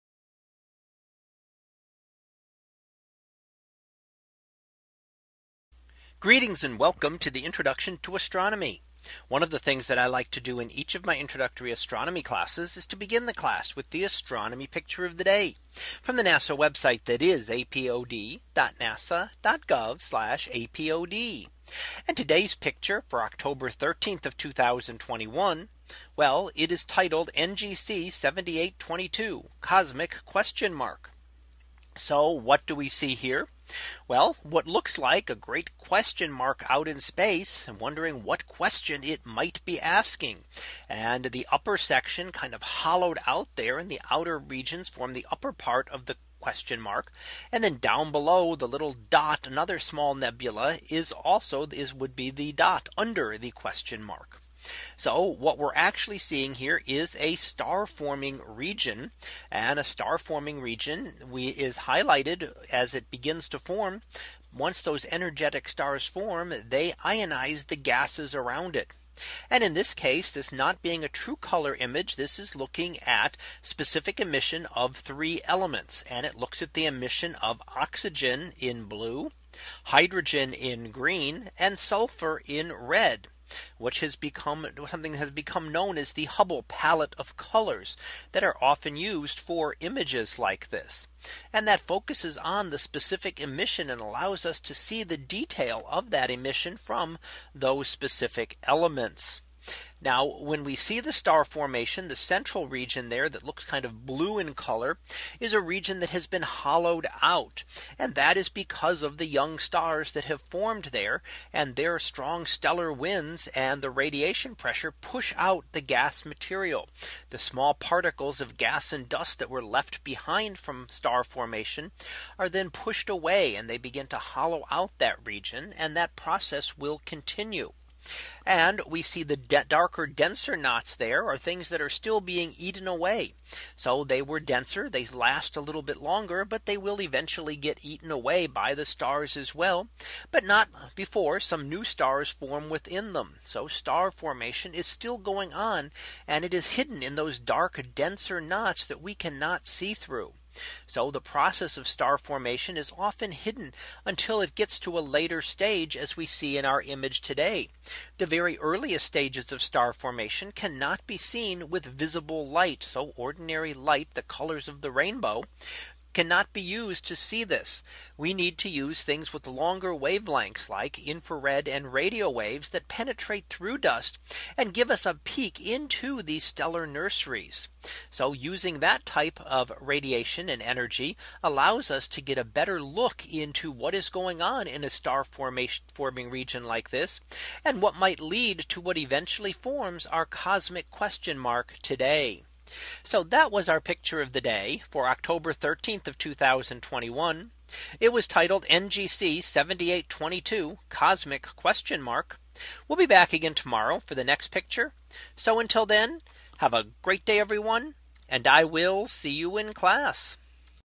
2021 October 13 - NGC 7822: Cosmic Question Mark
In today's image, we see an emission nebula in the shape of a question mark. This is a star forming region mapped in the light of three specific elements: oxygen, hydrogen, and sulfur.